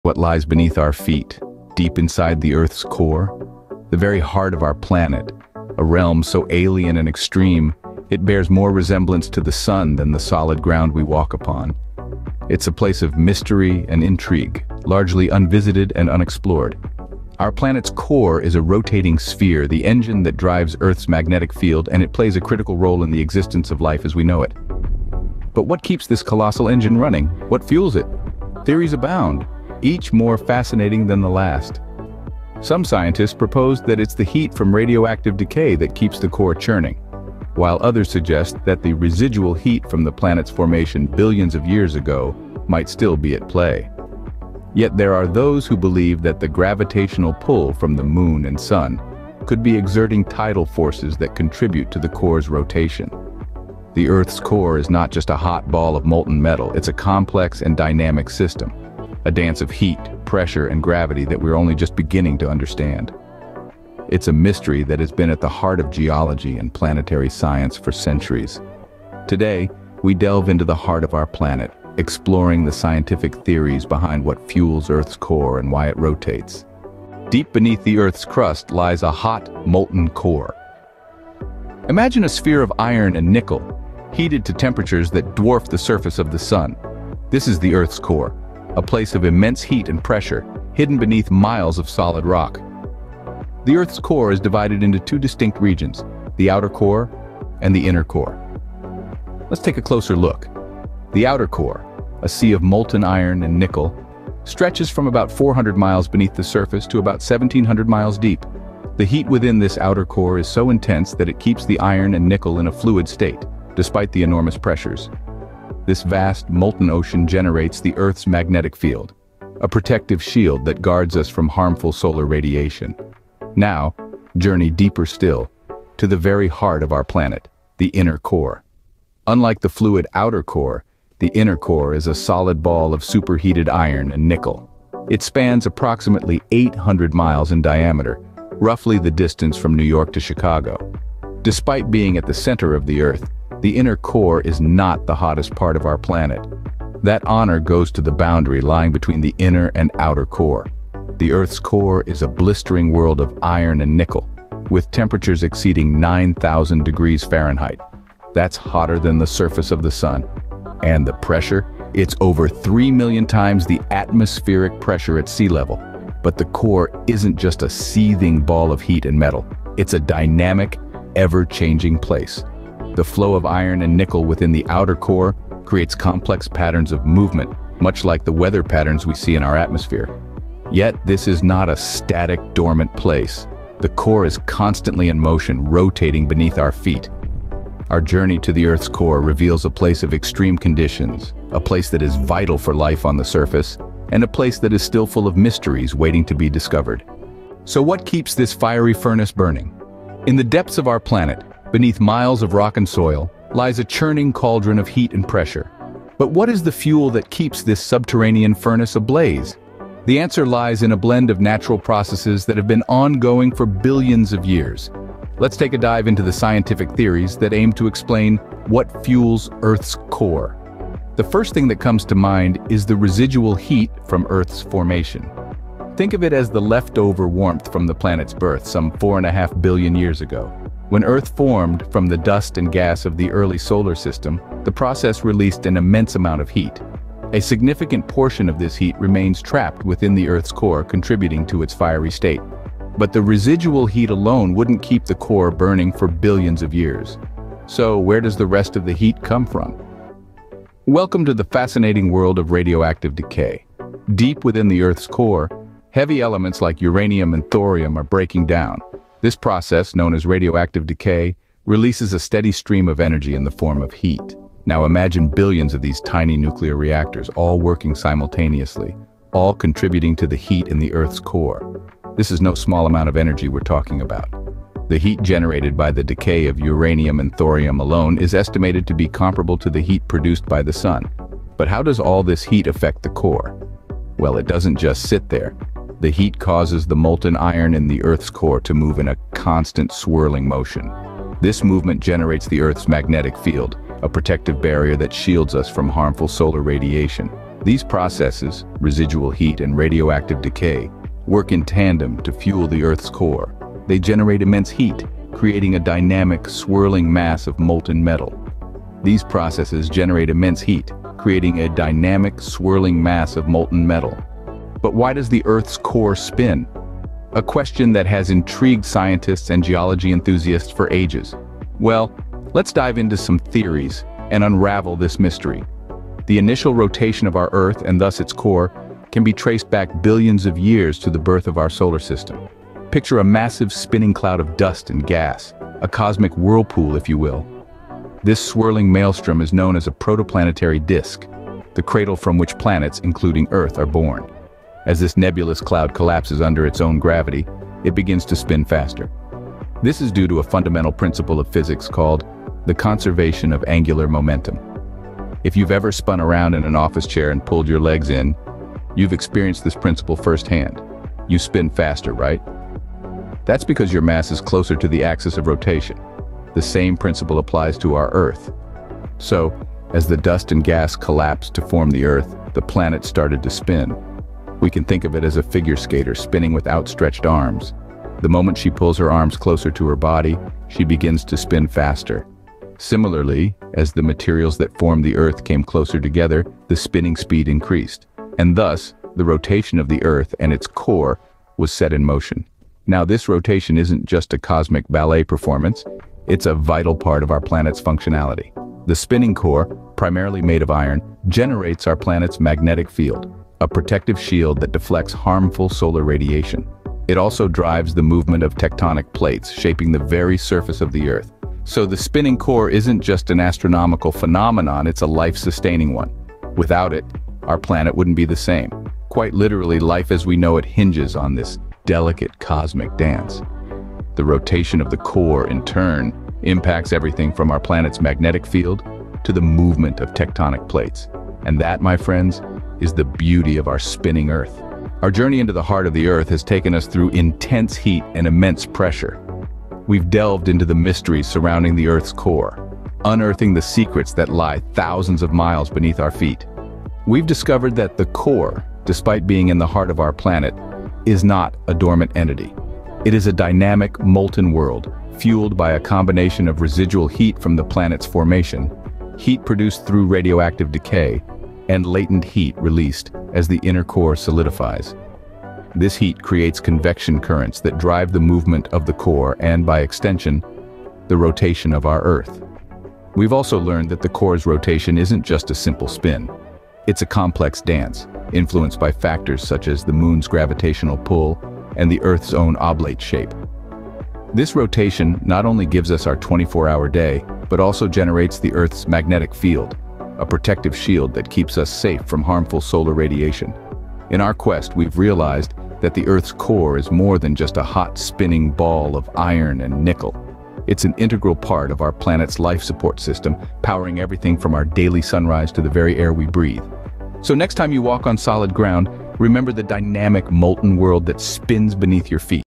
What Rotates The Earth's Core | AH Documentary
Greetings from our channel! We go on an intriguing journey to solve the mysteries surrounding the rotation of Earth, its core, and the strong forces that maintain it in motion in this engrossing video. Come along as we explore the unknown depths beneath our feet as we delve deeply into the inner workings of our planet. Learn about the inner workings that impact everything from geological activity to the Earth's magnetic field. Get ready to have your mind blown away by astonishing details and cutting-edge research. Take advantage of this fantastic chance to broaden your horizons and see the wonders of the world from a whole new angle. Together, let's plunge into the depths!

OUTLINE:

00:00:00 Opening the Earth's Core Mystery
00:01:18 Unearthing the Earth's Core
00:03:18 Fueling the Furnace
00:05:15 The Spinning Core
00:07:15 Summarizing the Journey